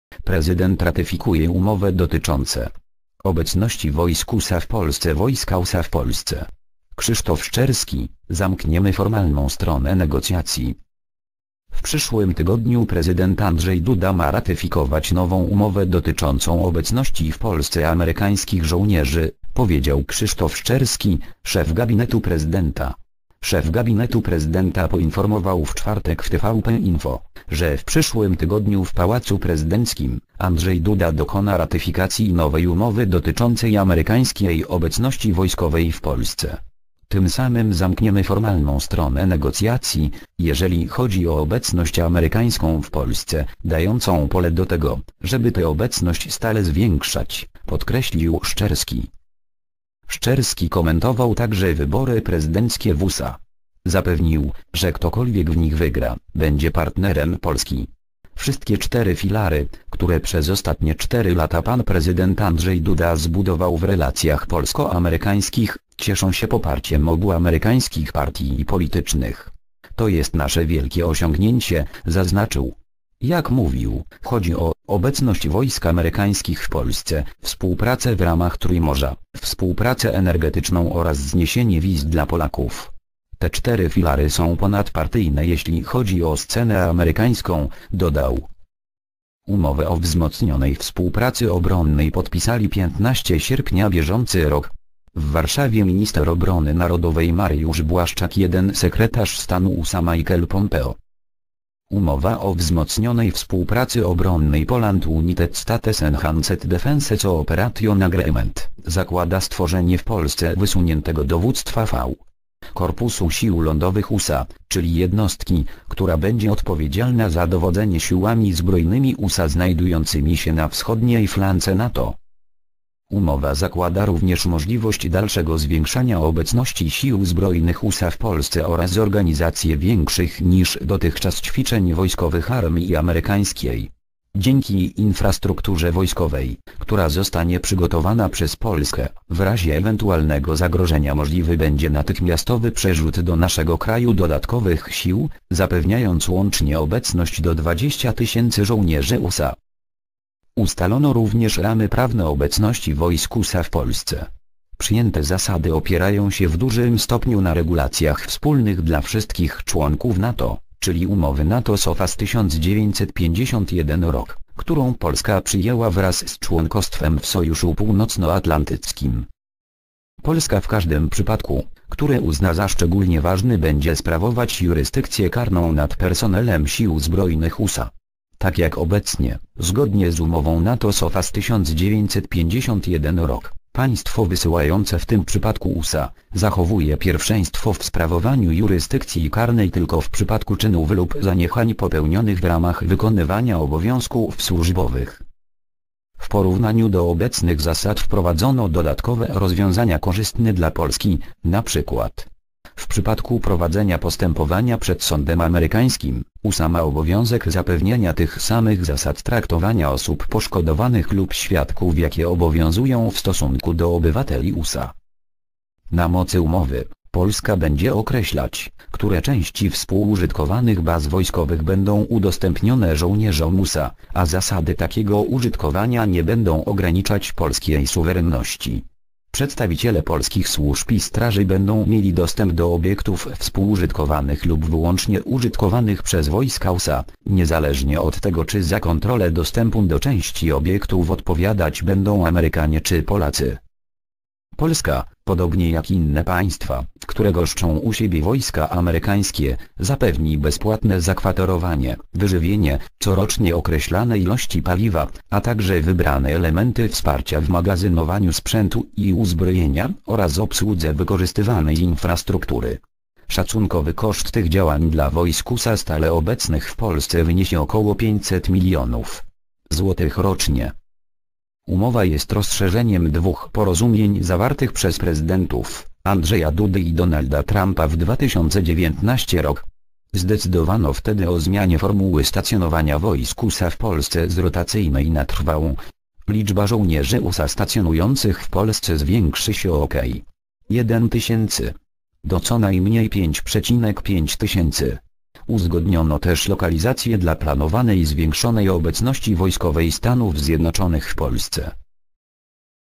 Prezydent ratyfikuje umowę dot. obecności wojsk USA w Polsce.
Wojska USA w Polsce. Krzysztof Szczerski: Zamkniemy formalną stronę negocjacji.

  (fot. Sean Gallup/Getty Images)